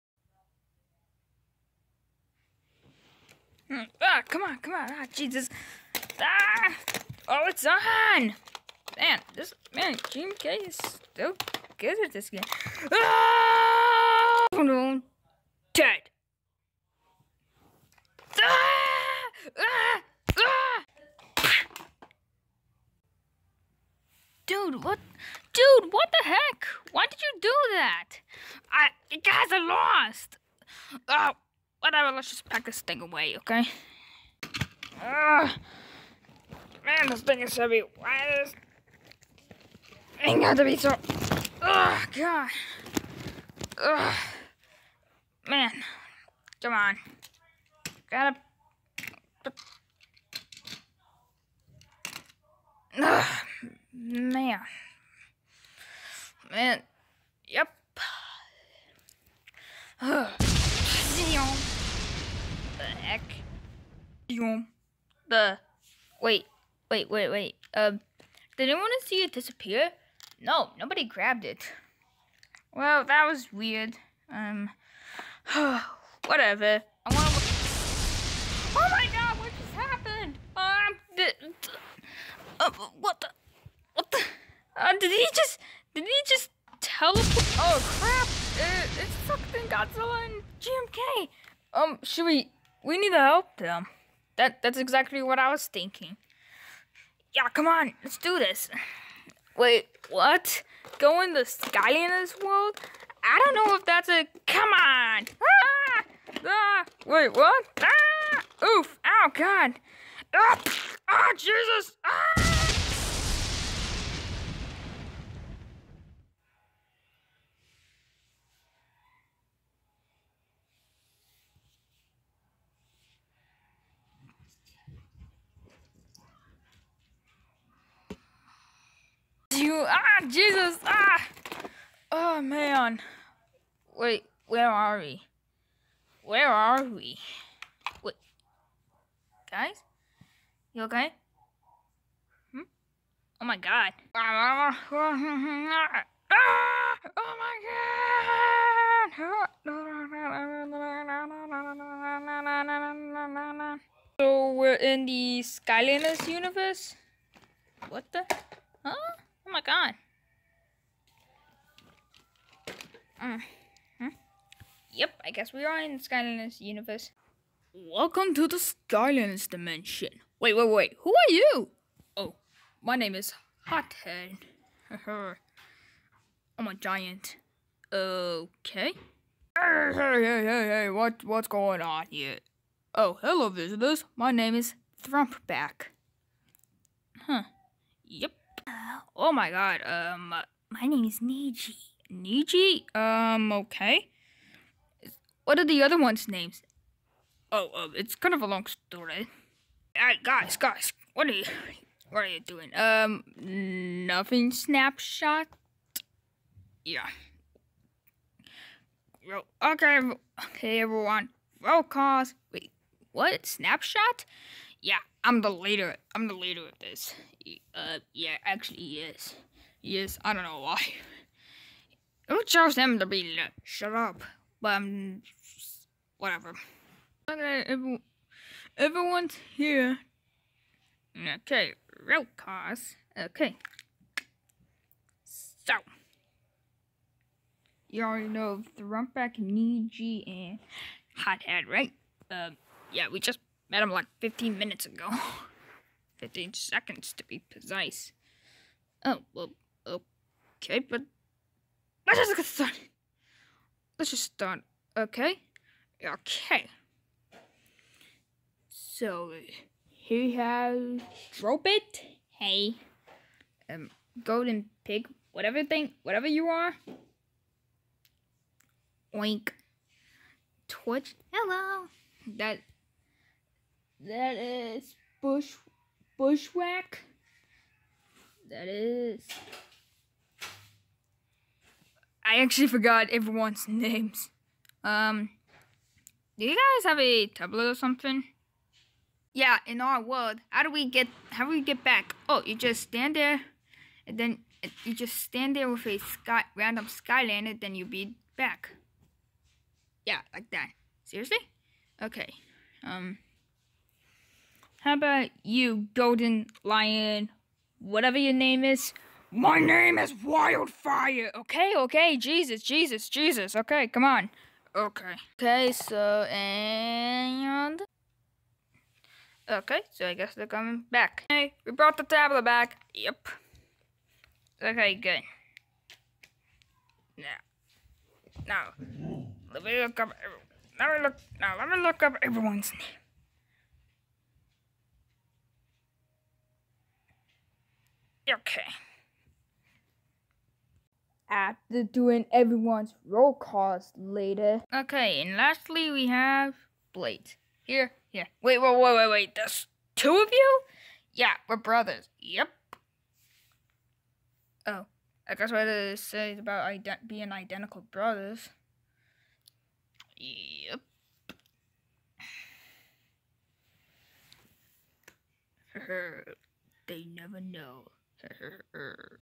Godzilla-verse #42: Godzilla vs Skylanders pt.1
Godzilla and his friends get teleported to the Skylanders universe